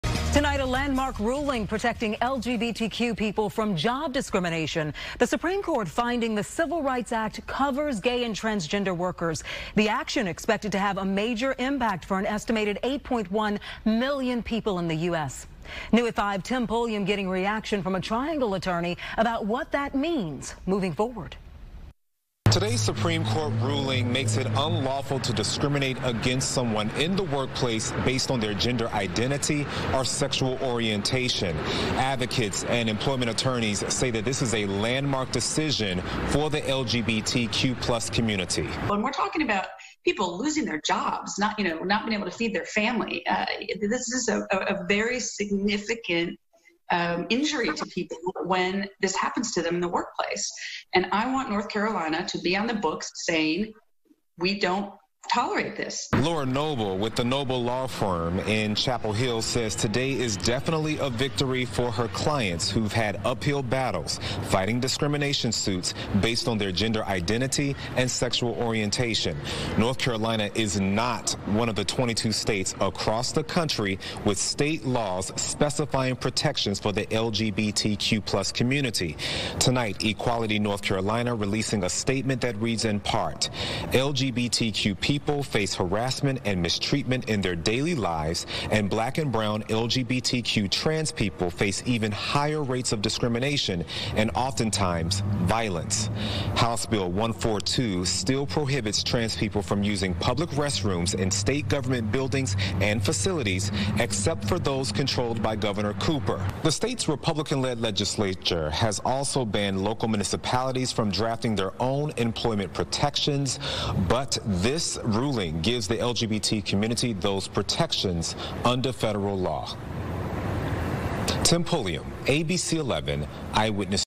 SCOTUS Ruling Protects LGBTQ Employees from Discrimination - Attorney Laura Noble on ABC 11 News
Employment Attorney Laura Noble explains how the Supreme Court's ruling on Bostock v. Clayton County, Georgia protects LGBTQ members from workplace discrimination based on their sexual orientation or gender identity, as well as how this specifically impacts employees in North Carolina.